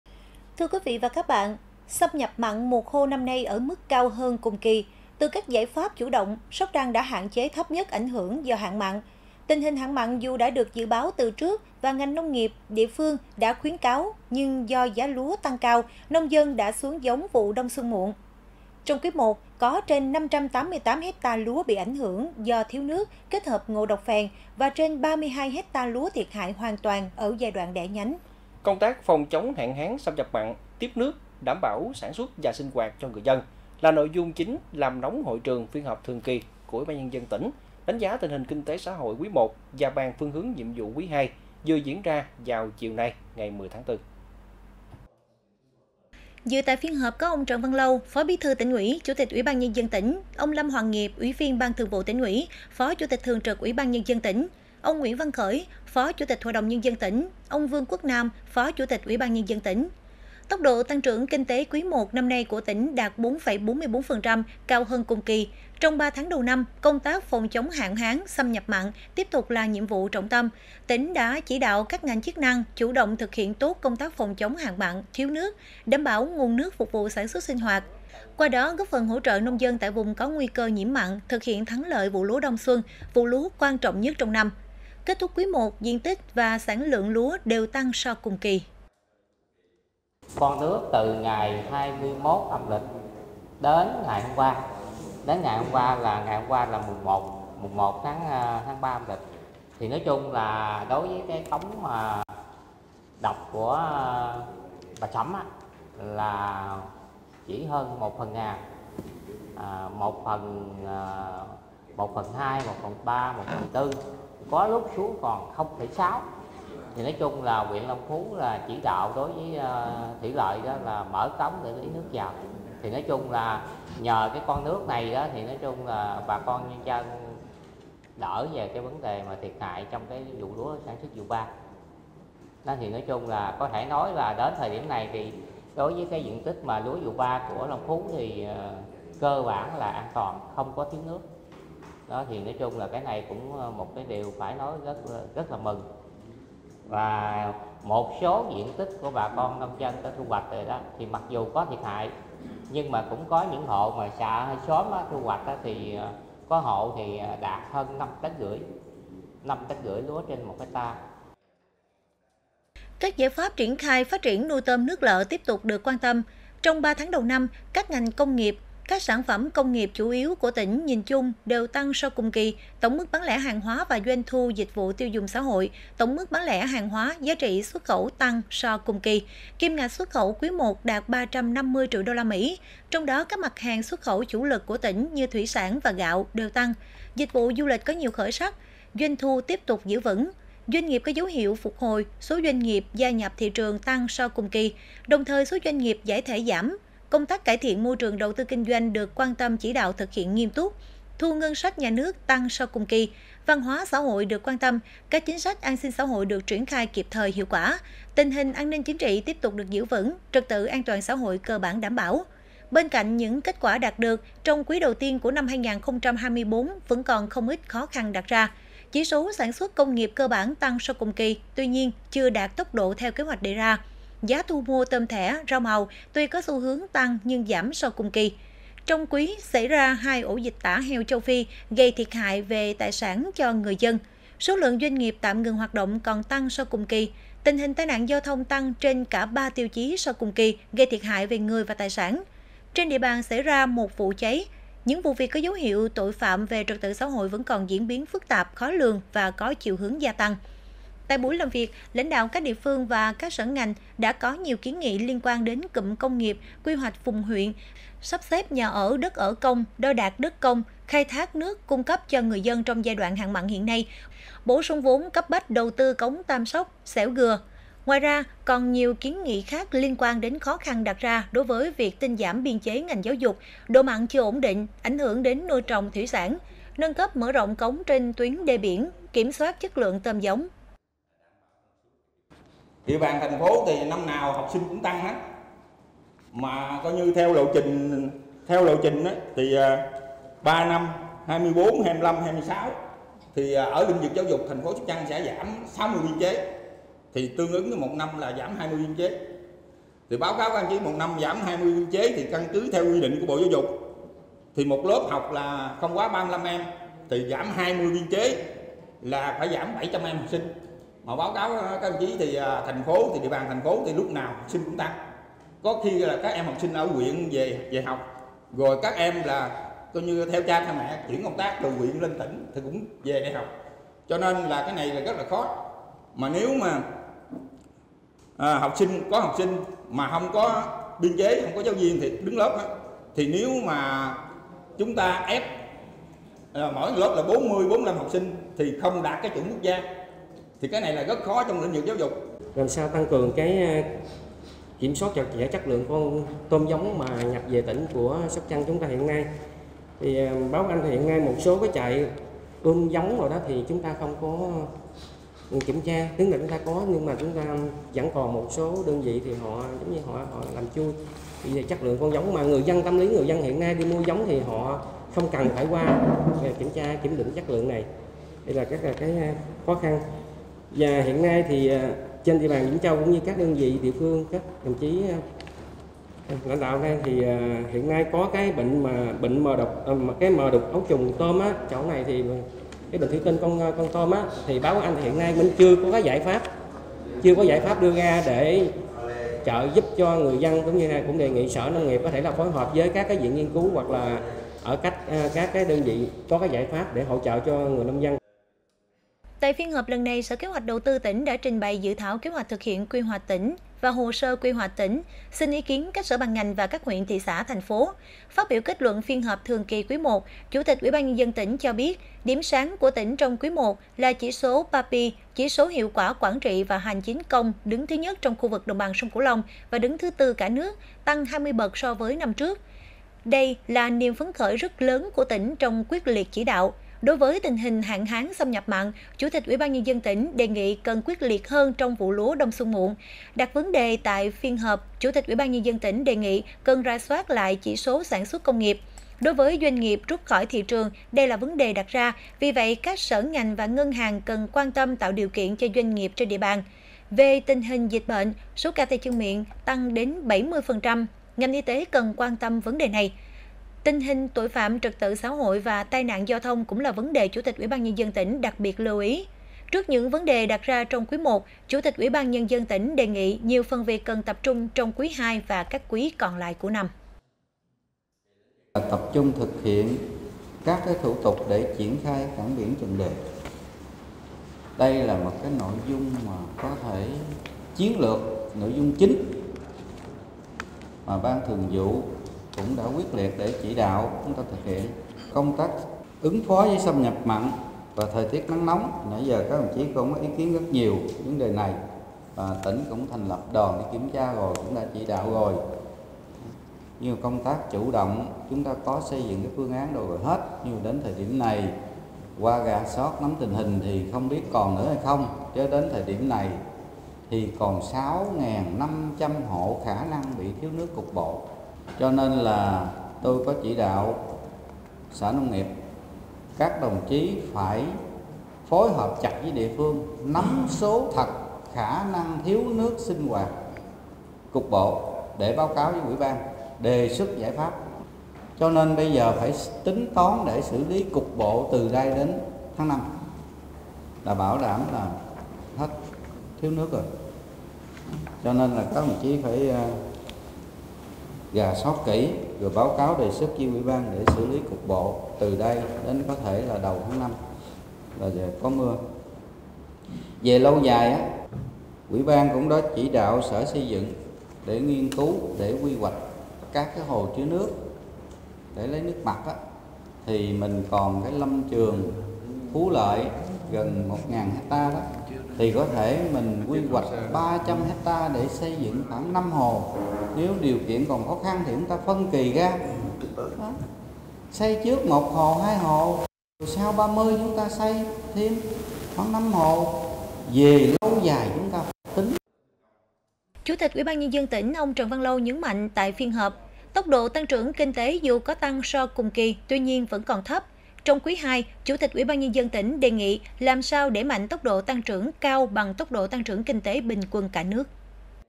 STV - Sóc Trăng quyết tâm đẩy mạnh tốc độ tăng trưởng tăng kinh tế
STV - Chiều ngày 10/4, UBND tỉnh Sóc Trăng tổ chức phiên họp thường kỳ của nhằm đánh giá tình hình kinh tế - xã hội quý I và bàn phương hướng nhiệm vụ quý II năm 2024.